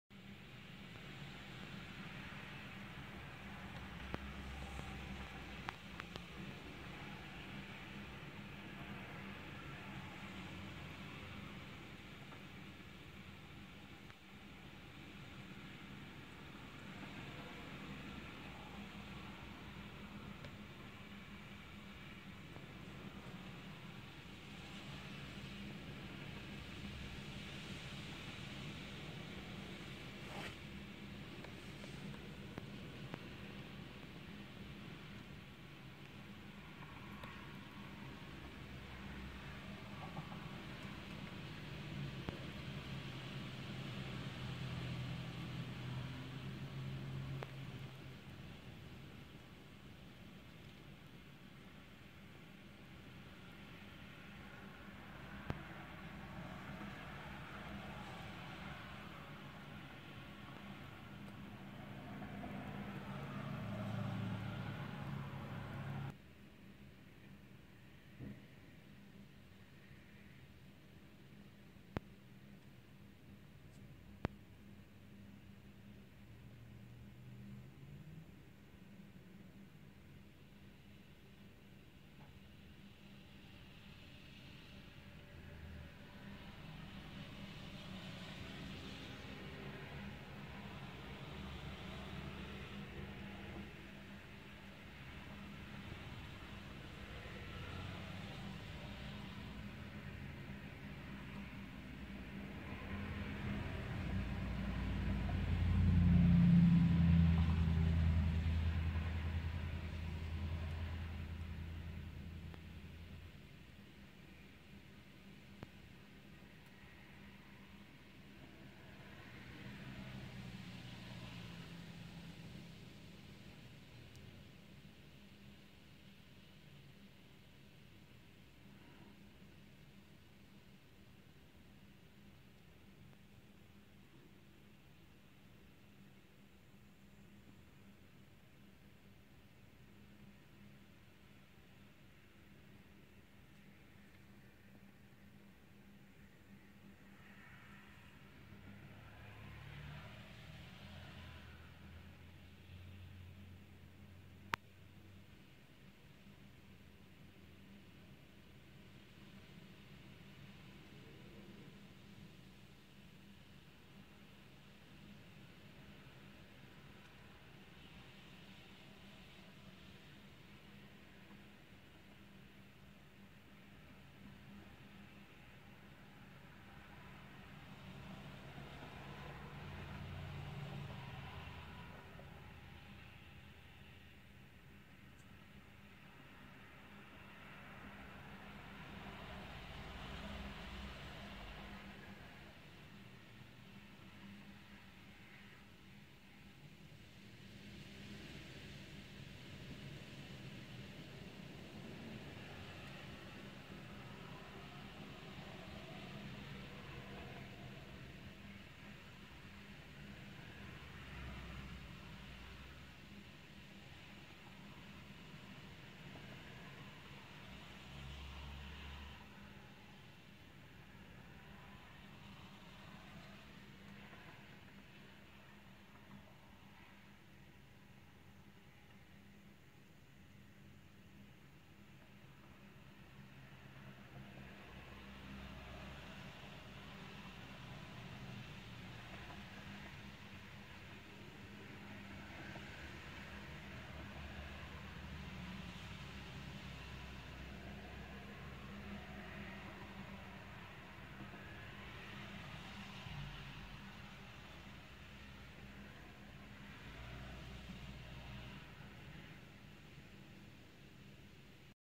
Here comes the snow...2018 (Snowfall part 1)
I recorded this snow fall today (Oct 20, 2018) in Batavia, Illinois. USA.